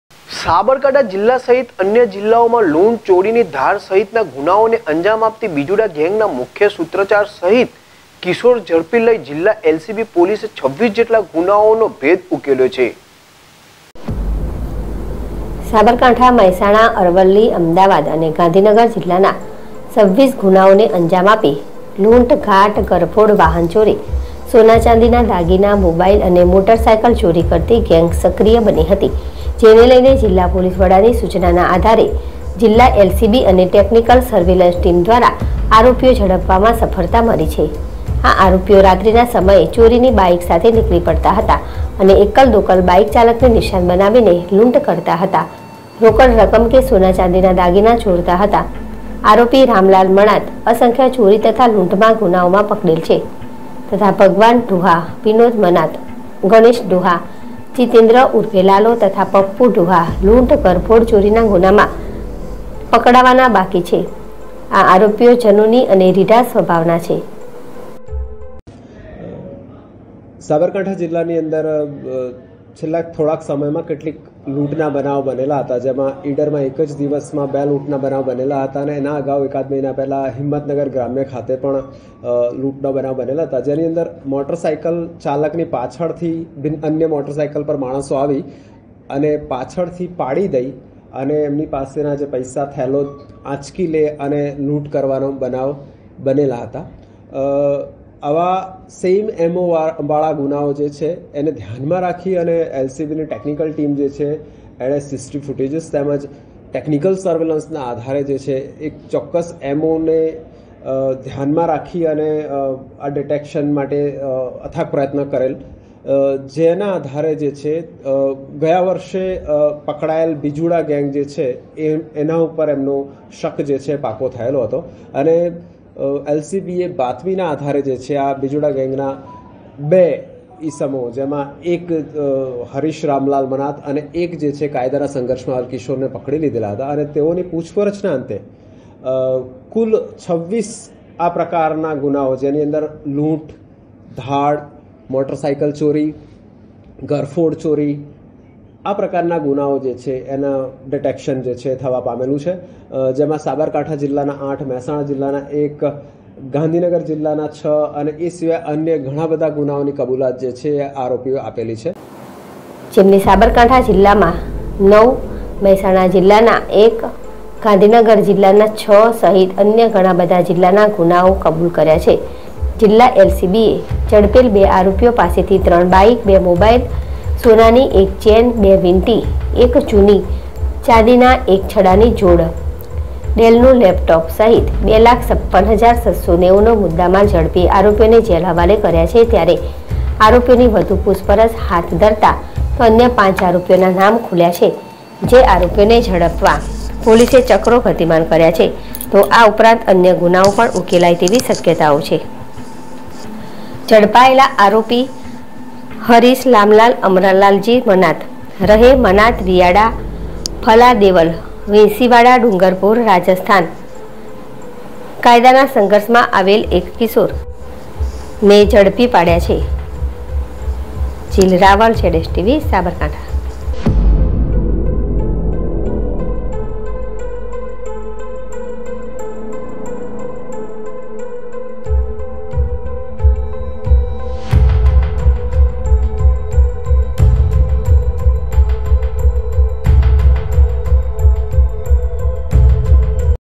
સાબરકાંઠા : રાજ્યમાં ધાડ, લૂંટ, ચોરીના ગુન્હાને અંજામ આપતી બિજુડા ગેંગના 2સાગરીતો ઝડપાયા
સાબરકાંઠા : રાજ્યમાં ધાડ, લૂંટ, ચોરીના ગુન્હાને અંજામ આપતી બિજુડા ગેંગના 2 સાગરીતો ઝડપાયા